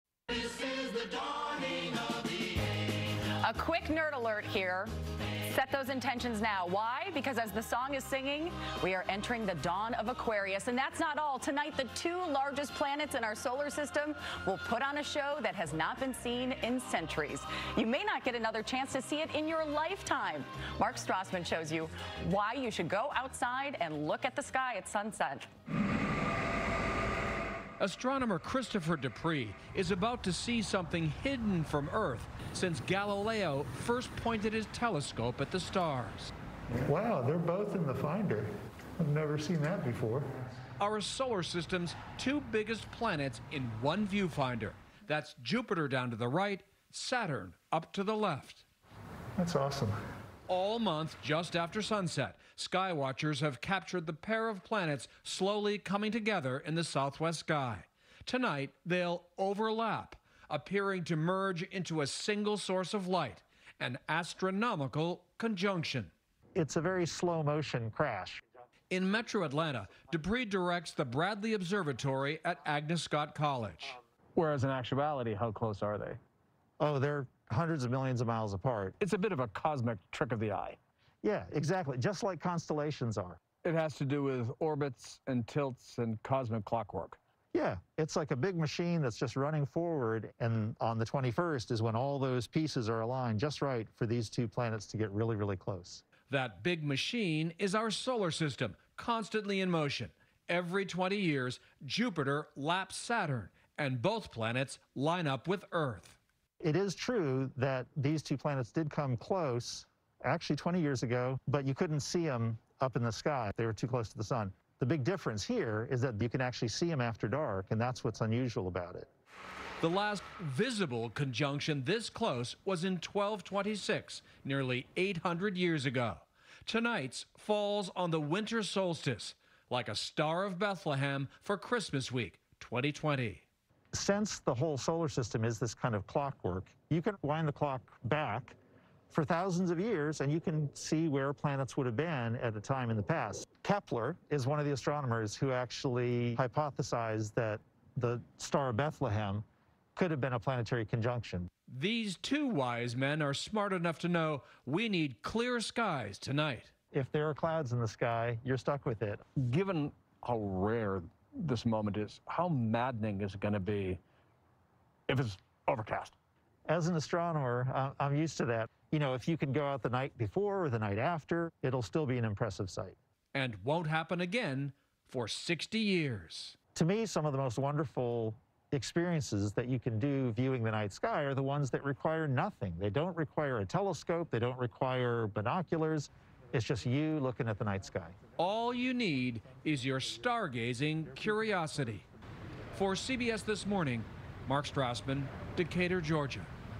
Christmas Star 2020: Jupiter, Saturn to meet in rare "great conjunction"
On Monday evening, the two largest planets in our solar system, Jupiter and Saturn, will appear to merge into a single source of light. It's the first visible conjunction of the giants in more than 800 years. Correspondent Mark Strassmann explains why we may not have another chance to see such an astronomical sight in your lifetime.